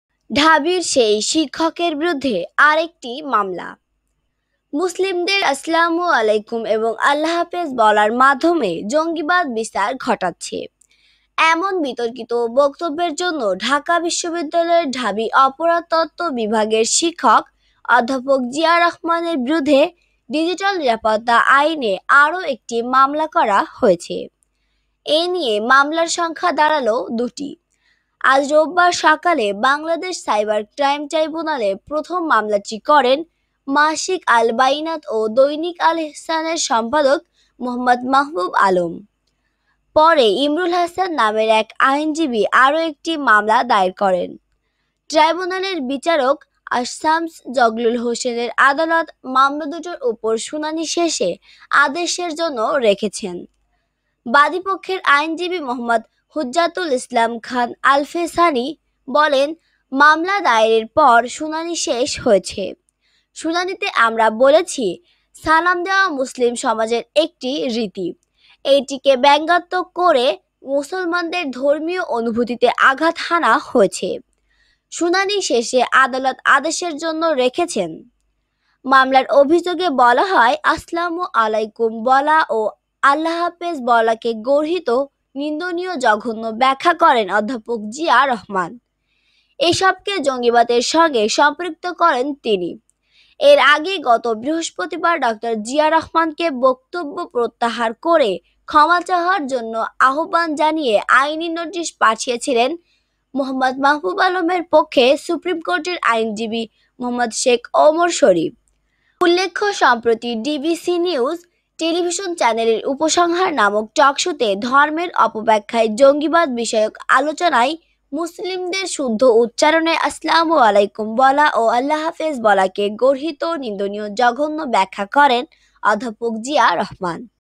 সালাম এবং আল্লাহ হাফেজ নিয়ে কটুক্তি করায় ঢাবির সেই শিক্ষকের বিরুদ্ধে মামলা | প্রফেসর জিয়াউর রহমান
ঢাবির সেই শিক্ষকের বিরুদ্ধে আরেকটি মামলা
  ‘মুসলিমদের আসসালামু আলাইকুম এবং আল্লাহ হাফেজ বলার মাধ্যমে জঙ্গিবাদ বিস্তার ঘটাচ্ছে’ এমন বিতর্কিত বক্তব্যের জন্য ঢাকা বিশ্ববিদ্যালয়ের (ঢাবি) অপরাধতত্ত্ব বিভাগের শিক্ষক অধ্যাপক জিয়া রহমানের বিরুদ্ধে ডিজিটাল নিরাপত্তা আইনে আরো একটি মামলা করা হয়েছে। এ নিয়ে মামলার সংখ্যা দাঁড়াল দুটি। আজ রোববার সকালে বাংলাদেশ সাইবার ক্রাইম ট্রাইব্যুনালে প্রথম মামলাটি করেন মাসিক আল বাইয়্যিনাত ও দৈনিক আল ইহসানের সম্পাদক মুহম্মদ মাহবুব আলম। পরে ইমরুল হাসান নামের এক আইনজীবী আরো একটি মামলা দায়ের করেন। ট্রাইব্যুনালের বিচারক আসসামছ জগলুল হোসেনের আদাল মামলা দুটোর ওপর শুনানি শেষে আদেশের জন্য রেখেছেন। বাদীপক্ষের আইনজীবী মুহাম্মদ হুজ্জাতুল ইসলাম খান আলফেসানী বলেন, ‘মামলা দায়েরের পর শুনানি শেষ হয়েছে। শুনানিতে আমরা বলেছি সালাম দেওয়া মুসলিম সমাজের একটি রীতি। এটিকে ব্যঙ্গাত্মক করে মুসলমানদের ধর্মীয় অনুভূতিতে আঘাত হানা হয়েছে। শুনানি শেষে আদালত আদেশের জন্য রেখেছেন।’ মামলার অভিযোগে বলা হয়, ‘আসসালামু আলাইকুম’ বলা ও ‘আল্লাহ হাফেজ’ বলাকে গর্হিত, নিন্দনীয়, জঘন্য ব্যাখ্যা করেন অধ্যাপক জিয়া রহমান। এসবকে জঙ্গিবাদের সঙ্গে সম্পৃক্ত করেন তিনি। এর আগে গত বৃহস্পতিবার  ড. জিয়া রহমানকে বক্তব্য প্রত্যাহার করে ক্ষমা চাওয়ার আহ্বান জানিয়ে আইনি নোটিশ পাঠিয়েছিলেন মুহম্মদ মাহবুব আলমের পক্ষে সুপ্রিম কোর্টের আইনজীবী মুহম্মদ শেখ ওমর শরীফ। উল্লেখ্য, সম্প্রতি ‘ডিবিসি নিউজ’ টেলিভিশন চ্যানেলের ‘উপসংহার’ নামক টক শোতে ‘ধর্মের অপব্যাখ্যায় জঙ্গিবাদ’বিষয়ক আলোচনায় মুসলিমদের শুদ্ধ উচ্চারণে ‘আসসালামু আলাইকুম’ বলা ও ‘আল্লাহ হাফেজ’ বলাকে গর্হিত, নিন্দনীয়, জঘন্য ব্যাখ্যা করেন অধ্যাপক জিয়া রহমান।